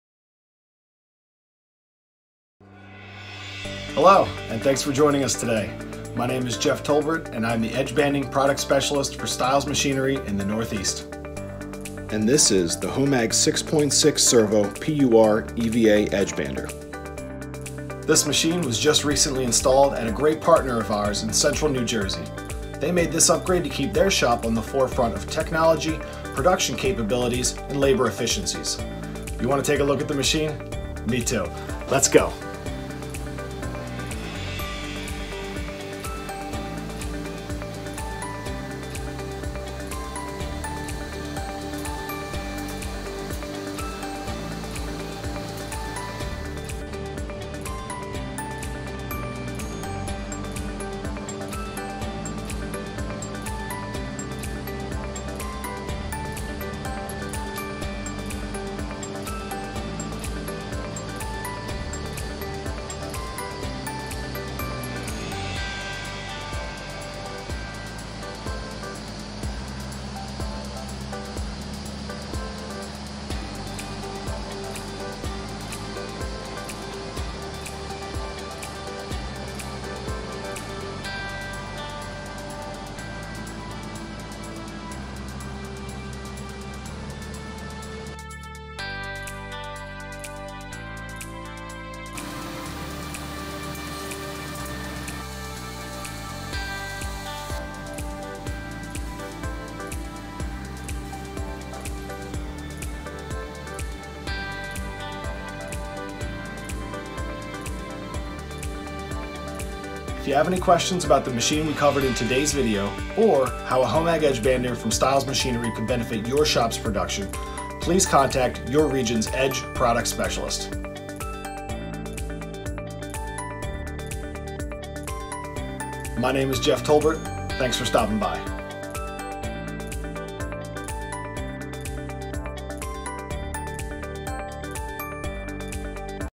HOMAG EDGETEQ 6.6 Servo PUR/EVA Edge Bander
In this video, I review stations and other production capabilities of the HOMAG EDGETEQ 6.6 Servo PUR/EVA Edge Bander from Stiles Machinery.